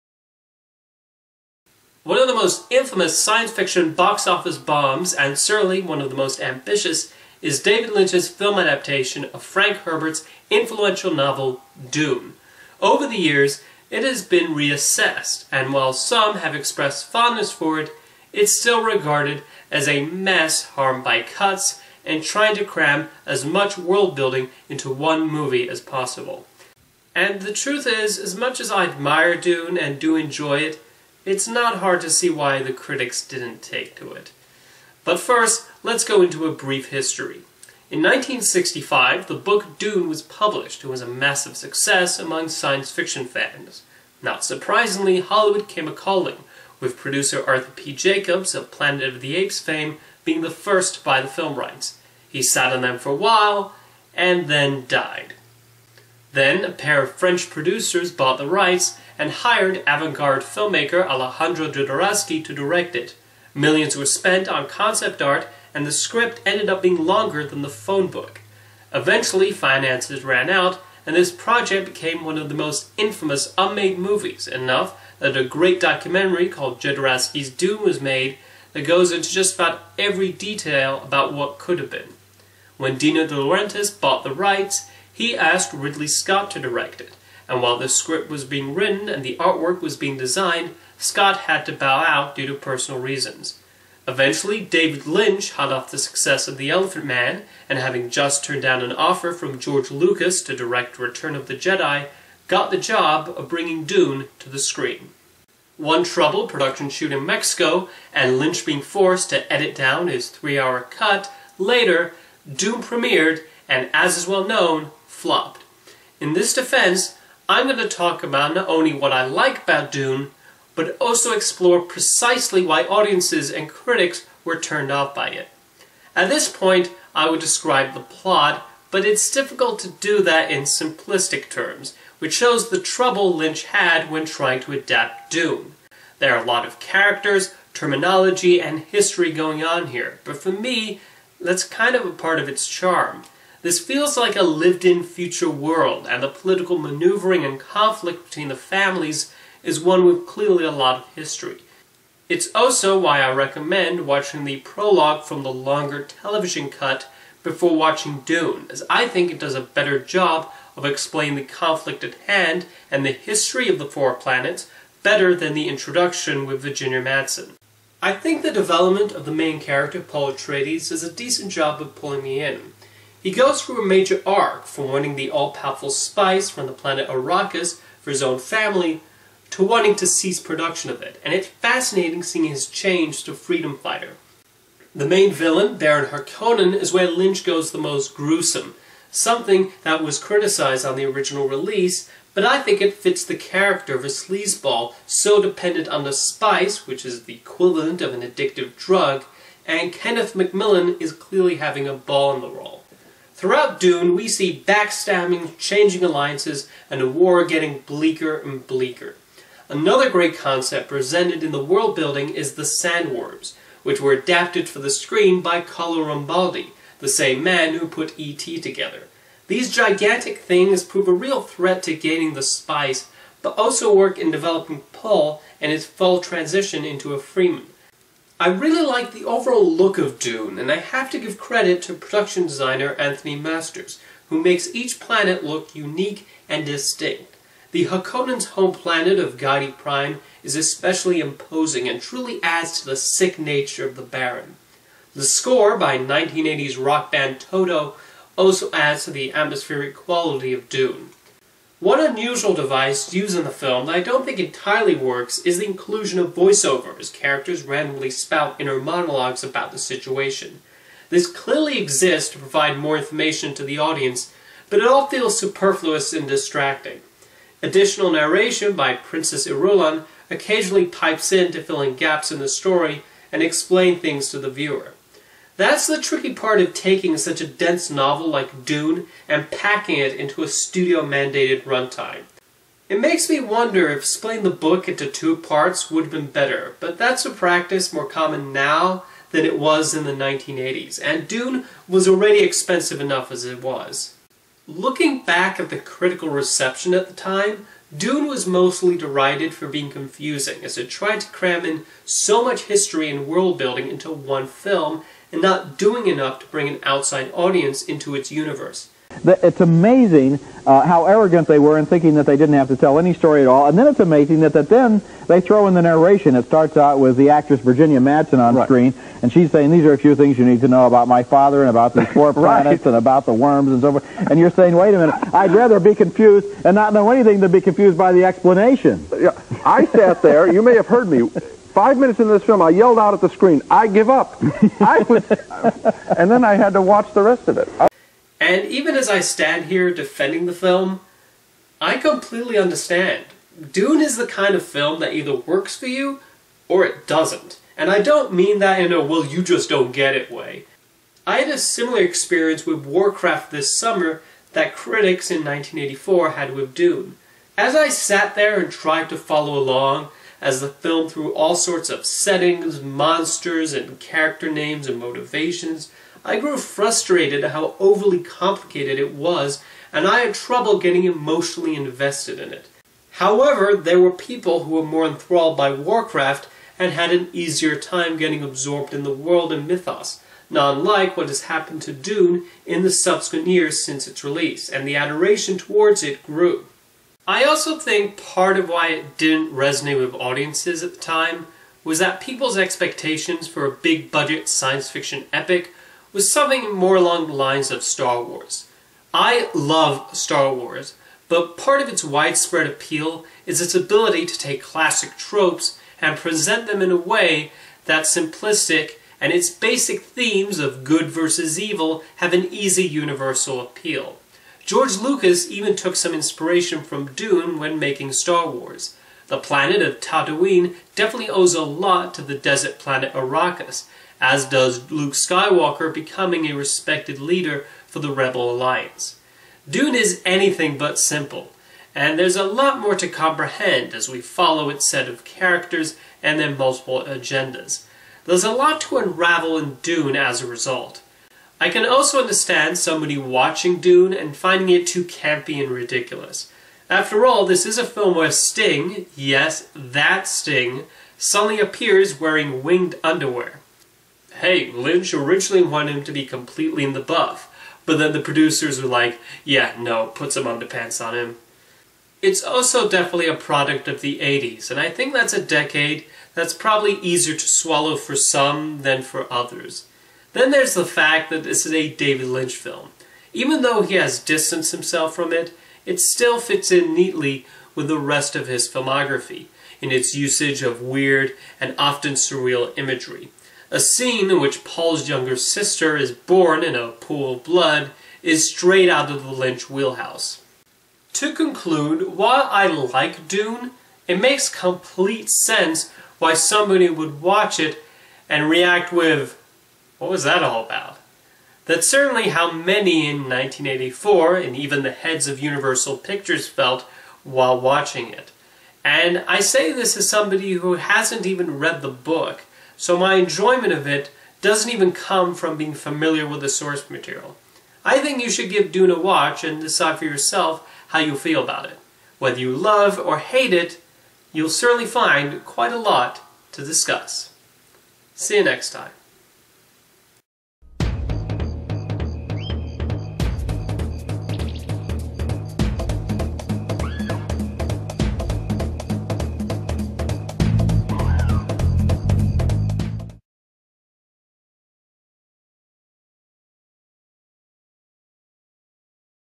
In Defense of David Lynch's "Dune"
I look at David Lynch's ambitious attempt at adapting Frank Herbert's dense science-fiction classic "Dune" into a major motion picture and analyse why critics hated it in 1984.